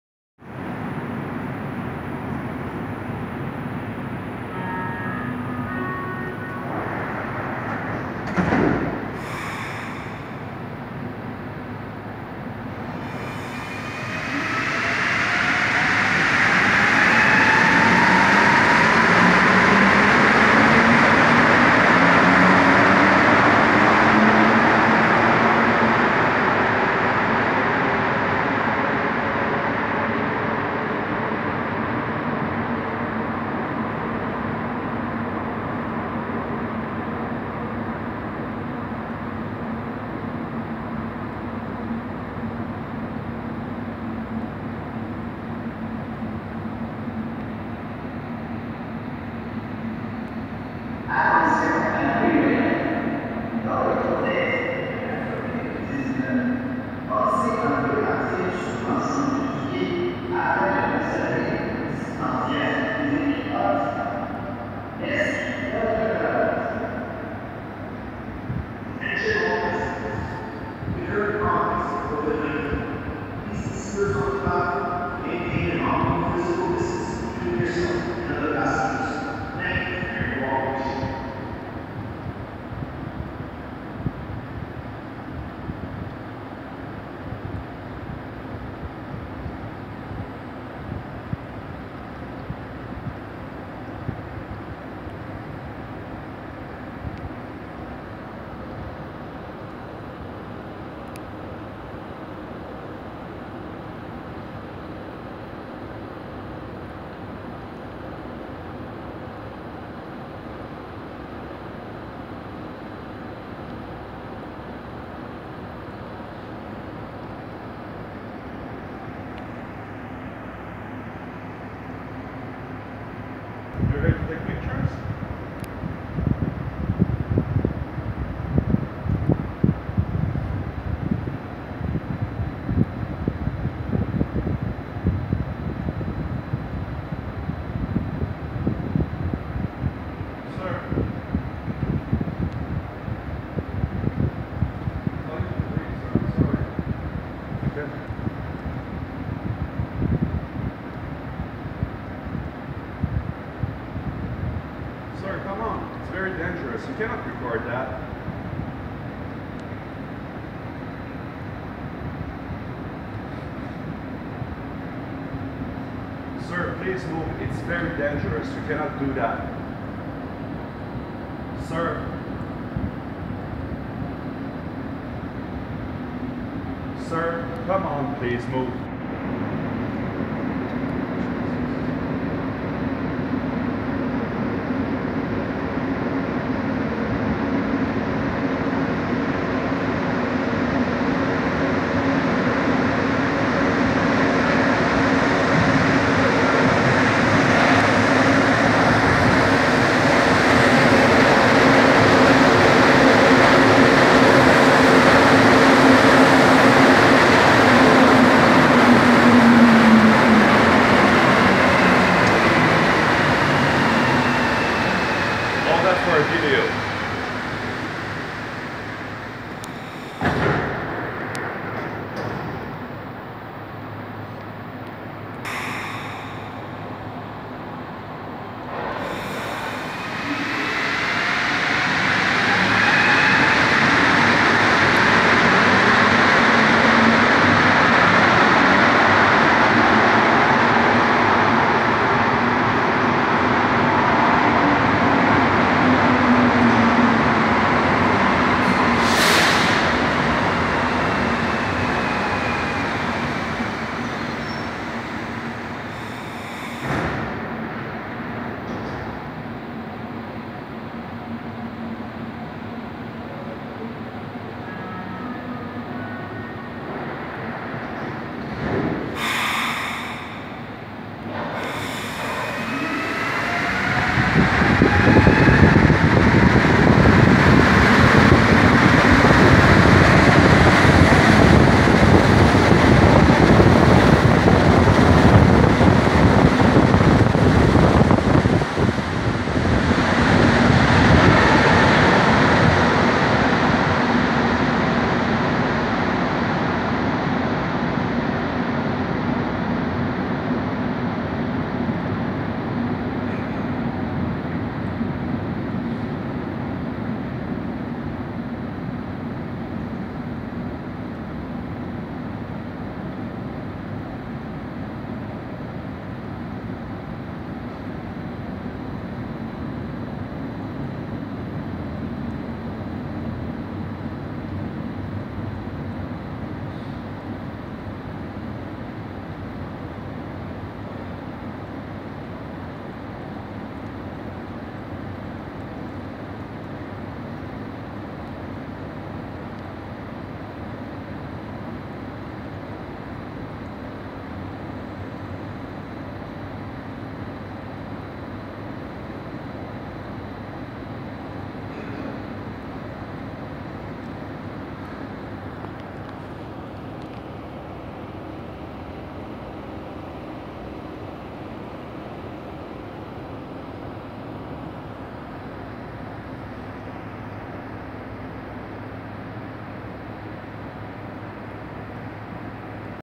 Station Crémazie (iii) *
Ligne orange du métro Montréalais.

* J'ai été bouscoulé par un tel guerrier de justice social en signalant des virtues inutiles à 2:59! J'ai enlevé la tranche de 3,5 secondes, car je lui ai instruit de ne me toucher JAMAIS, parce qu'il m'avait tiré par mon dos..puis on le voit à bord après la fermeture des portes.
* I was assaulted by some nauseatingly virtue signaling SJW at 2:59 when he yanked me from grabbing ahold of my napsack, and at whom I had to holler to NEVER touch me; hence, I've erased that 3½-second moment from this recording, although you can glimpse the geezer on board the arriving train when it departs.